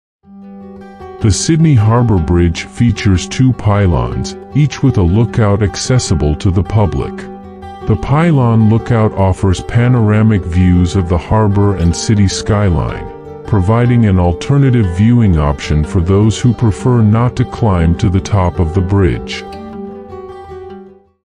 07 🌆🌉 Sydney Harbour Bridge Facts
07 🌆🌉 Sydney Harbour Bridge Facts

🌆🌉 The Sydney Harbour Bridge, located in Sydney, Australia, is an iconic landmark and a major tourist attraction. This magnificent steel arch bridge, affectionately known as the "coat hanger," spans across Sydney Harbour, connecting the bustling city with the picturesque North Shore. Standing proudly alongside the Sydney Opera House, the bridge forms an iconic backdrop that has come to symbolize the city's identity. 🌆🌉 The Sydney Harbour Bridge is not only a feat of engineering marvel, but it also offers breathtaking views and unforgettable experiences. Visitors can witness the spectacular New Year's Eve fireworks display from the bridge or embark on the thrilling Bridge Climb, ascending to its summit for panoramic vistas of the cityscape and the sparkling harbor below. As the heart of Sydney's transportation network, the bridge serves as a vital link for commuters and a symbol of connectivity. Its majestic arch and graceful design have earned it a well-deserved place in the hearts of locals and visitors alike. 🌆🌉 The Sydney Harbour Bridge is more than just a functional structure; it is a cherished emblem of Sydney's vibrant spirit and a testament to human ingenuity.